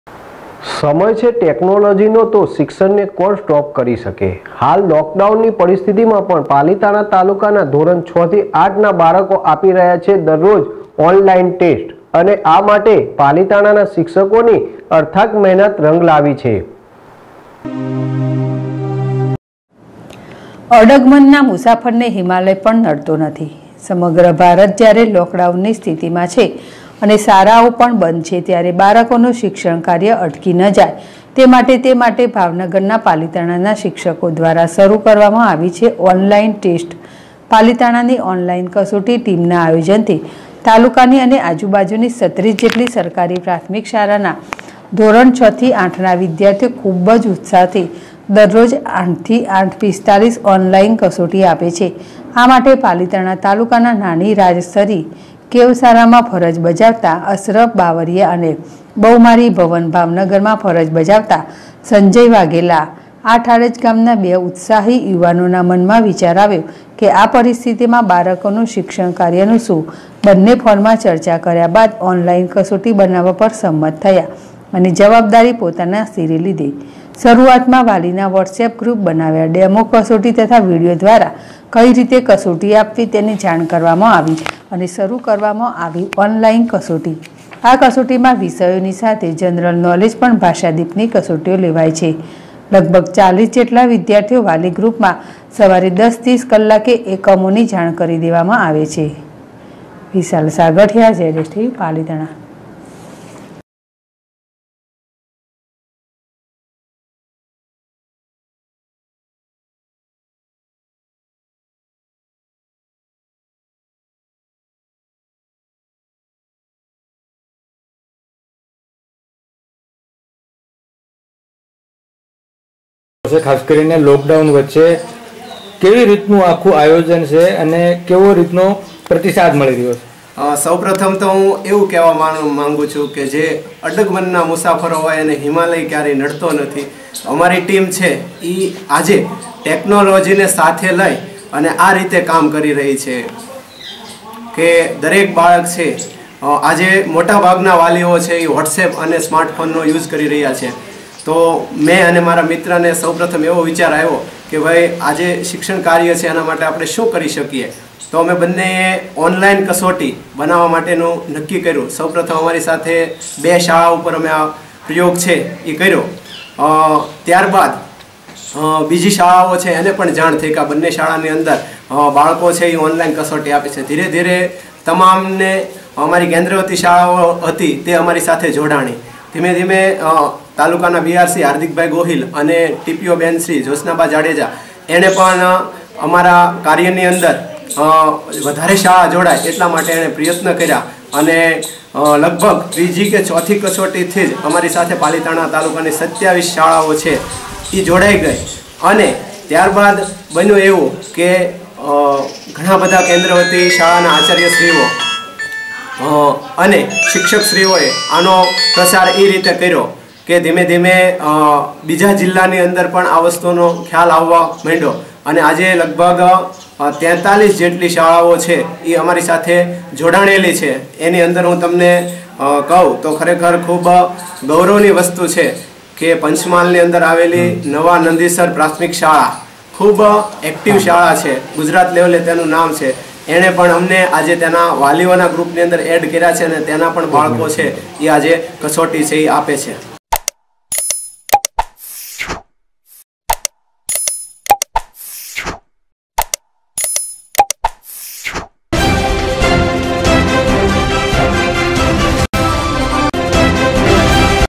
પાલીતાણા હાલ લોકડાઉન ની પરિસ્થિતિ માં પણ પાલીતાણા તાલુકાના ધો.6 થી 8 ના બાળકો આપી રહ્યા છે
પાલીતાણા સમય છે ટેક્નોલૉજી નો તો શિક્ષણ ને કોણ સ્ટોપ કરી શકે ?